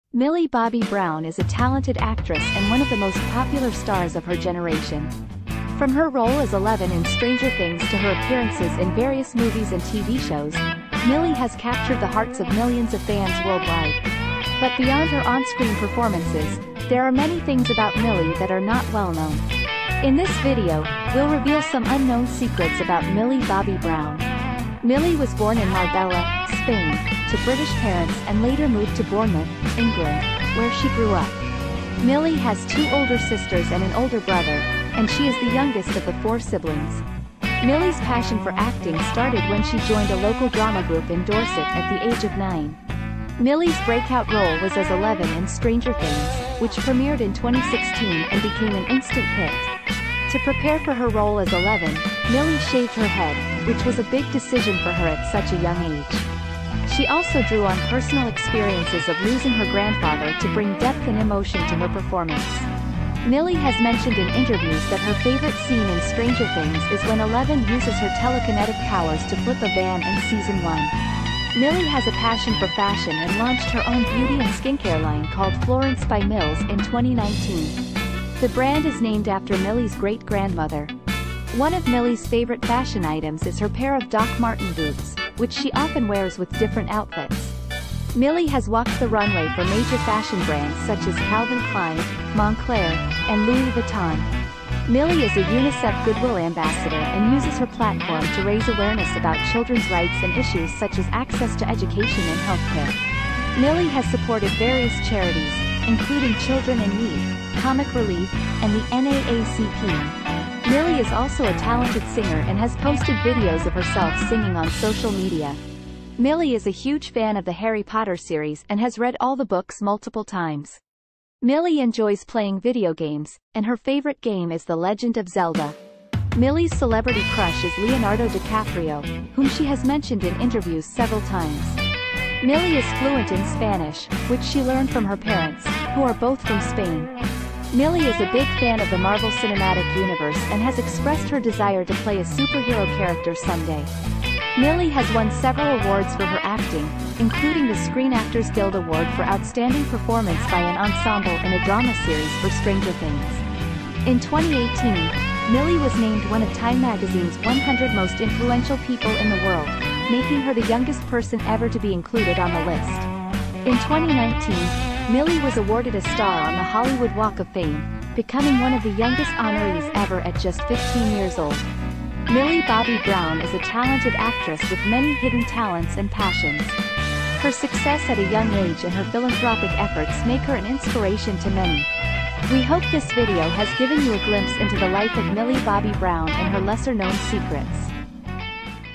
Unbelievable Millie Bobby Brown Truths You Won't Believe!
If you're a Millie Bobby Brown fan, then you'll love these Unbelievable Millie Bobby Brown Truths You Won't Believe! In this video, we'll share some of the most interesting and shocking details about Millie Bobby Brown's life and career.

From her early days as a child actor to her recent role in the hit TV show "Stranger Things", Millie Bobby Brown is one of the most popular stars in Hollywood today. In this video, we'll take a look at some of the most shocking facts about Millie Bobby Brown's life and career!